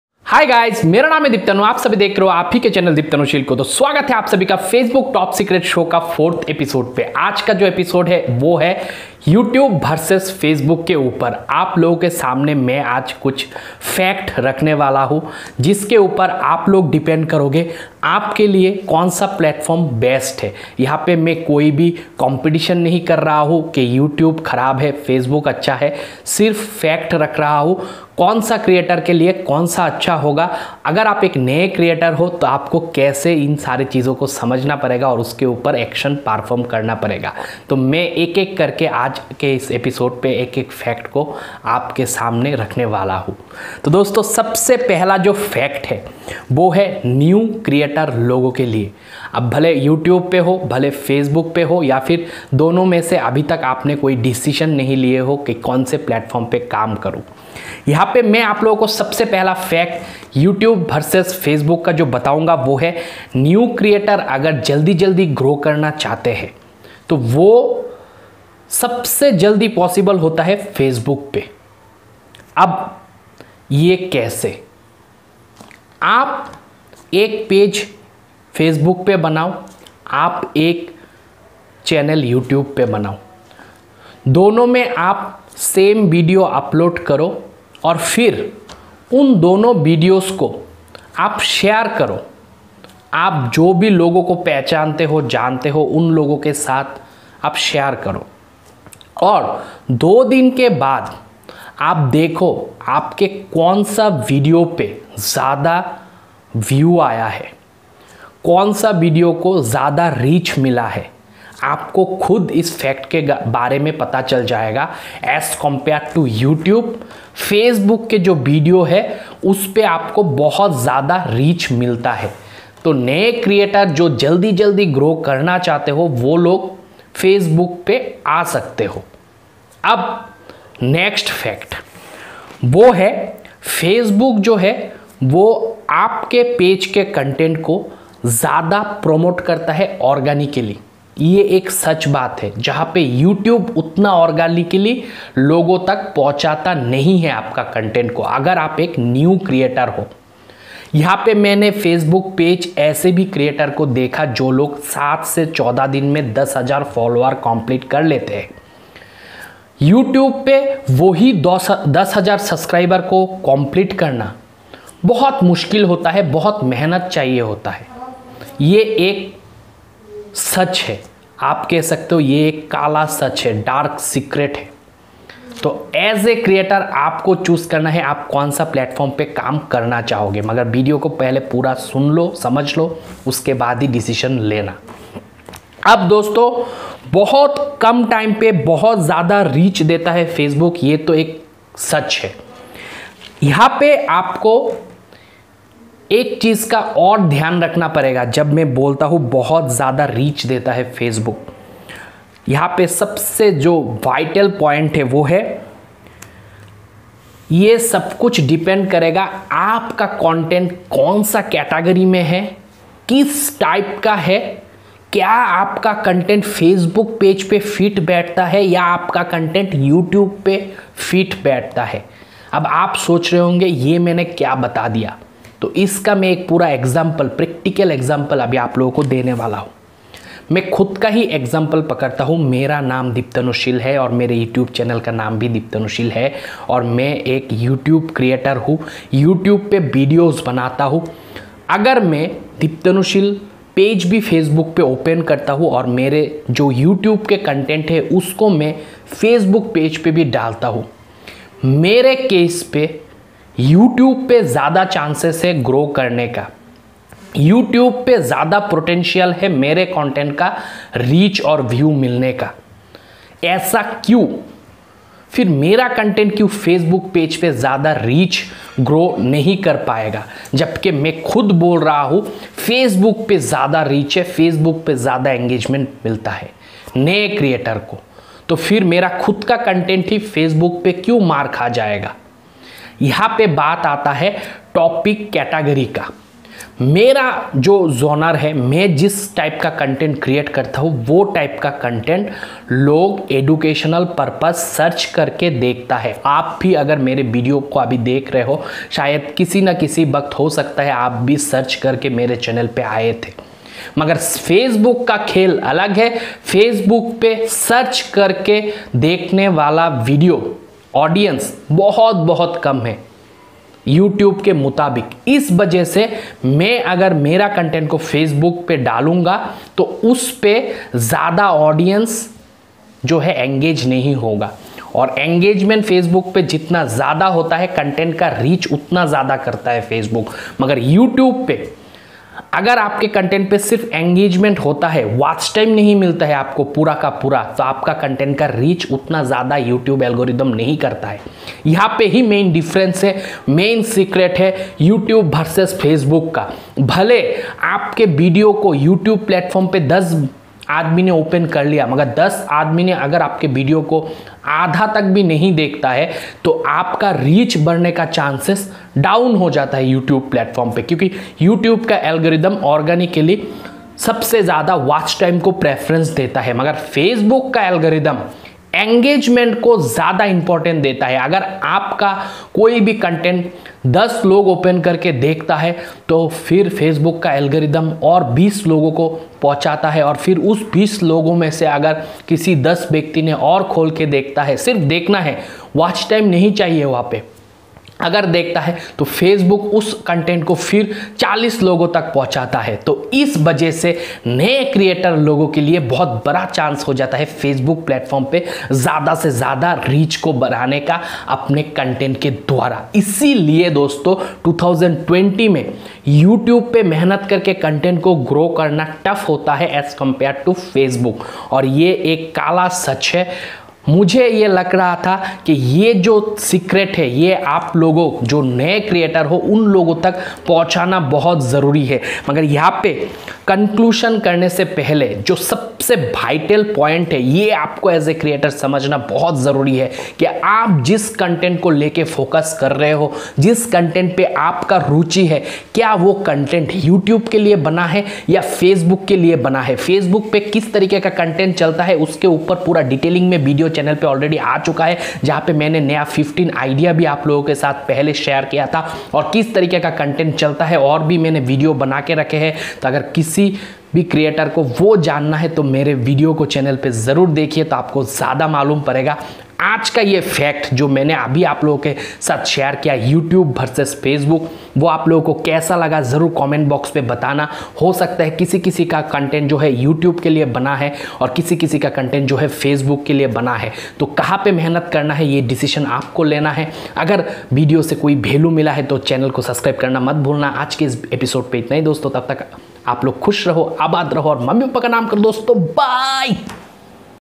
YOUTUBE vs FACEBOOK WHICH GROWS FAST? | DIPTANU SHIL
True Facebook of Youtube vs Facebook? As a new creator where you get more reach, more engagement and more grow fast.